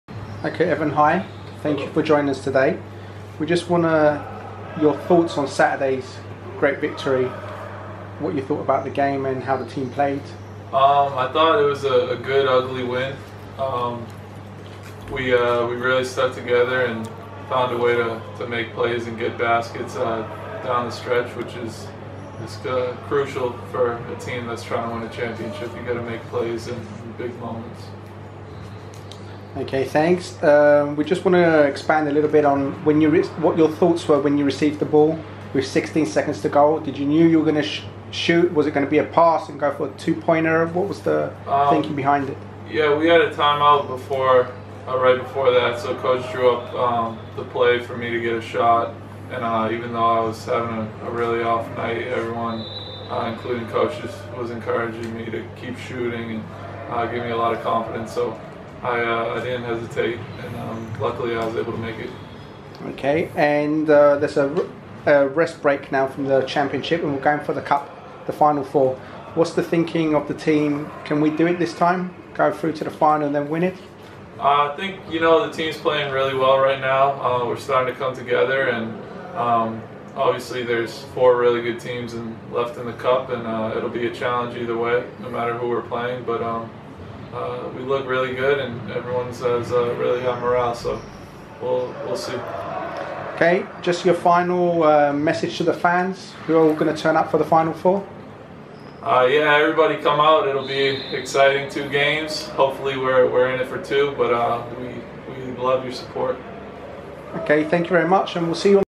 EVAN SMOTRYCZ INTERVIEW
about the great victory over Apollon (55-57) on Friday 05/02/2016